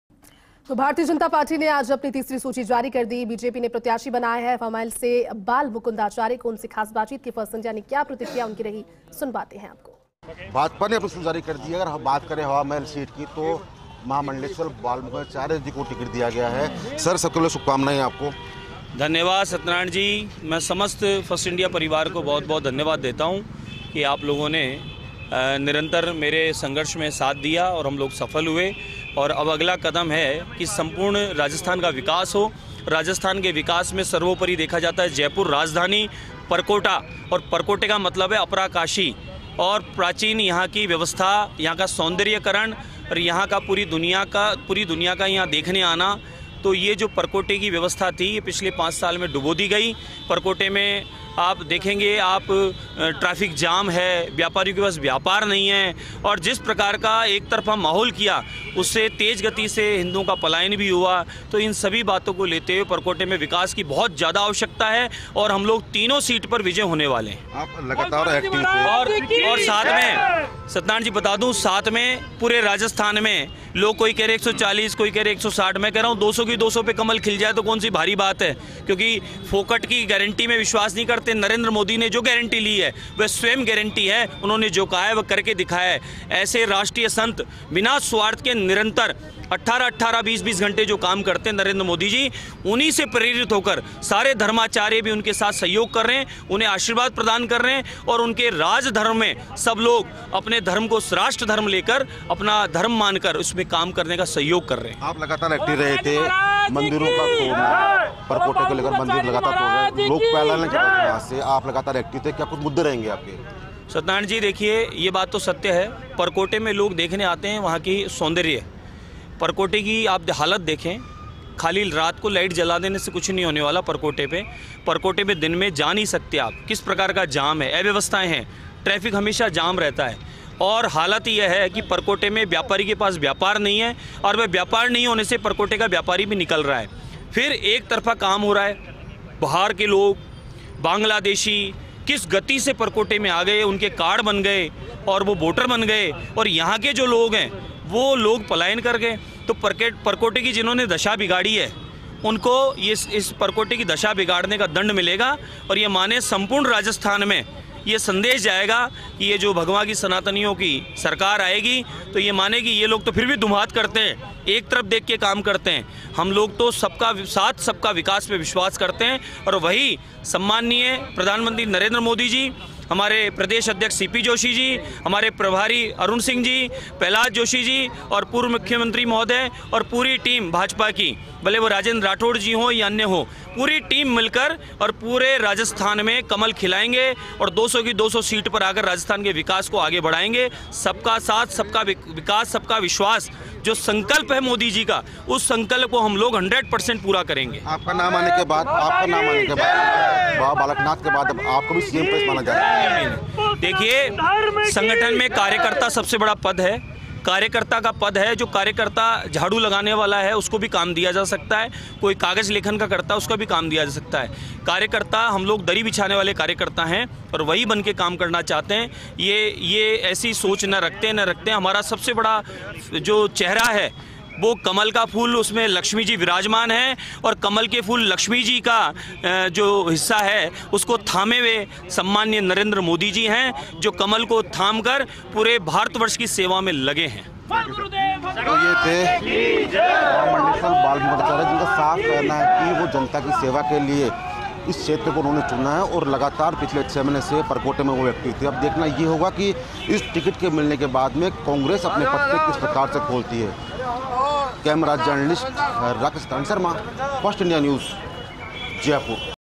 हवामहल से BJP उम्मीदवार बालमुकुंद आचार्य से खास बातचीत | Rajasthan Election 2023
About this Video:

भाजपा ने जारी की प्रत्याशियों की तीसरी सूची,पिलानी से राजेश दहिया, खेतड़ी से धर्मपाल गुर्जर,सीकर से रतनलाल जलधारी, खण्डेला से सुभाष मील,विराटनगर से कुलदीप धनखड़, जमवारामगढ़ से महेंद्रपाल मीणा,हवामहल से आचार्य बालमुकुन्दाचार्य, किशनगढ़ बास से रामहेत सिंह यादव,बहरोड़ से जसवंत सिंह यादव, रामगढ़ से जय आहूजा, राजगढ़-लक्ष्मणगढ़ से बन्नाराम मीणा, कठूमर से रमेश खिंची,कामां से नौक्षम चौधरी, नदबई से जगत सिंह
रामगंजमंडी से मदन दिलावर, अन्ता से कंवरलाल मीणा,किशनगंज से ललित मीणा, बारां-अटरू से सारिका चौधरी,बयाना से बच्चू सिंह बंशीवाल, बसेड़ी से सुखराम कोली को भाजपा ने बनाया प्रत्याशी,   | 02 November 2023